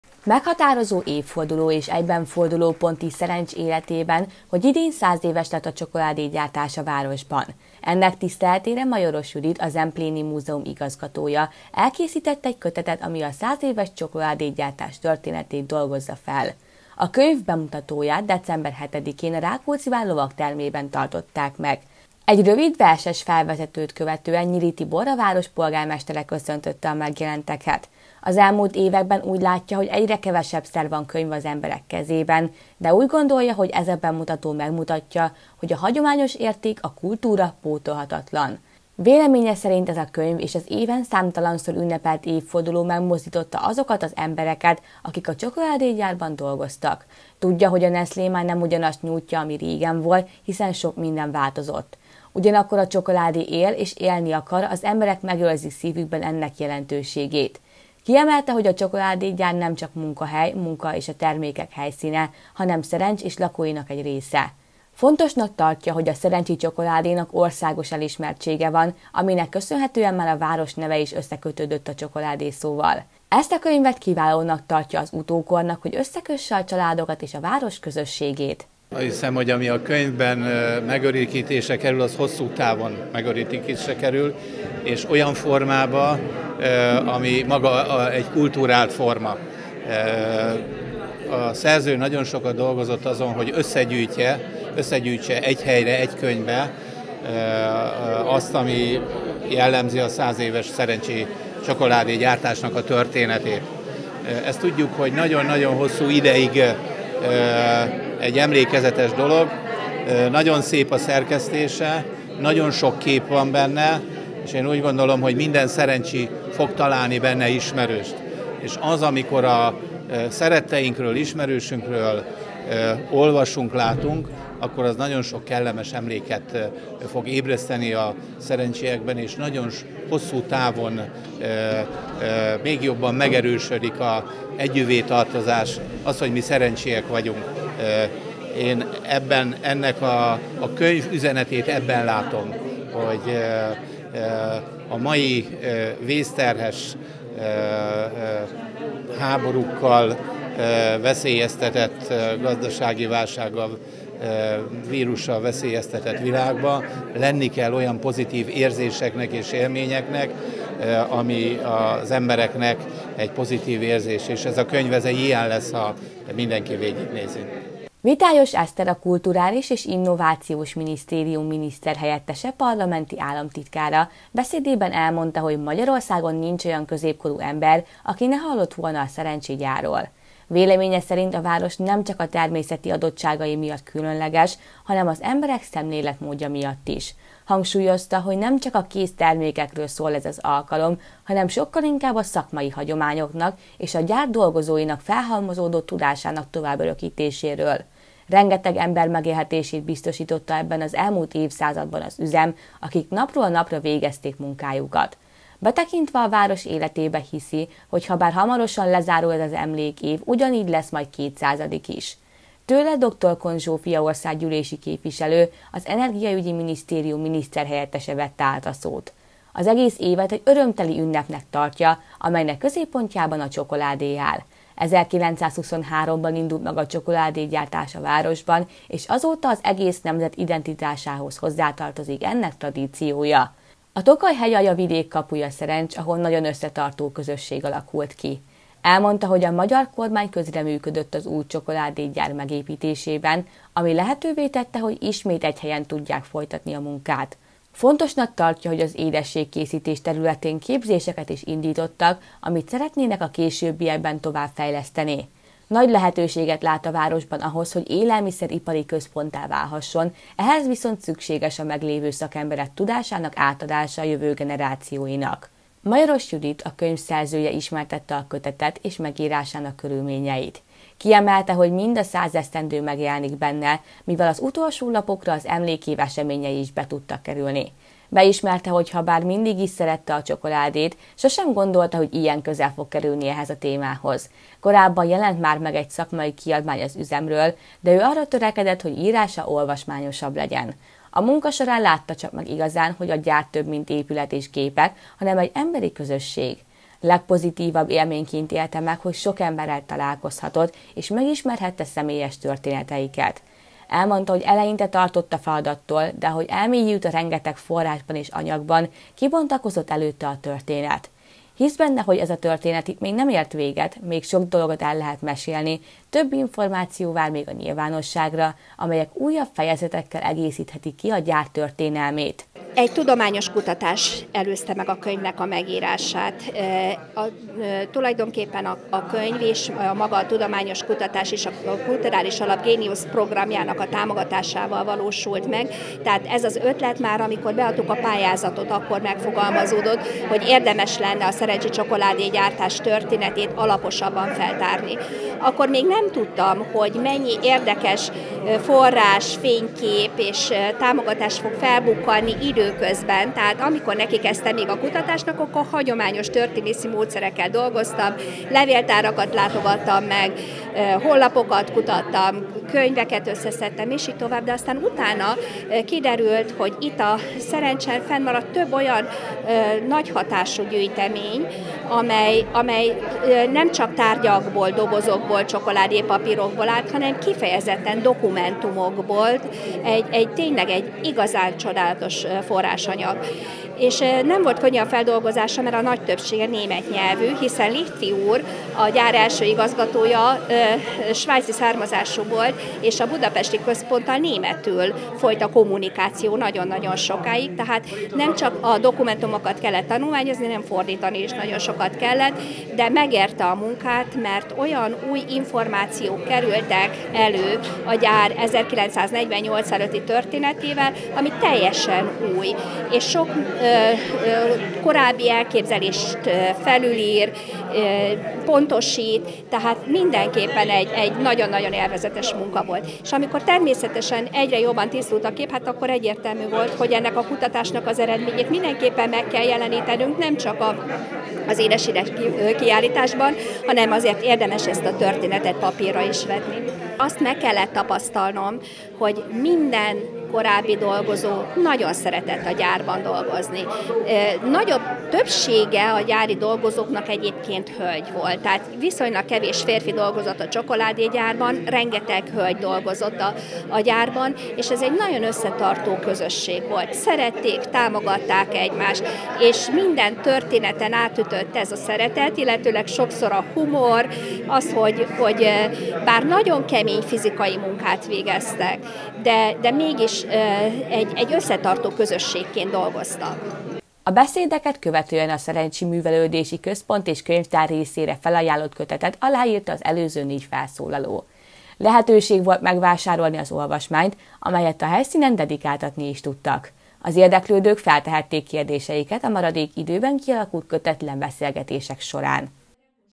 A SZERENCSI CSOKOLÁDÉGYÁRTÁS 100 ÉVE
Meghatározó évforduló, és egyben fordulópont is Szerencs életében, hogy idén száz éves lett a csokoládégyártás a városban. Ennek tiszteletére Majoros Judit, a Zempléni Múzeum igazgatója elkészített egy kötetet, ami a száz éves csokoládégyártás történetét dolgozza fel. A könyv bemutatóját december 7-én a Rákóczi-vár Lovagtermében tartották meg.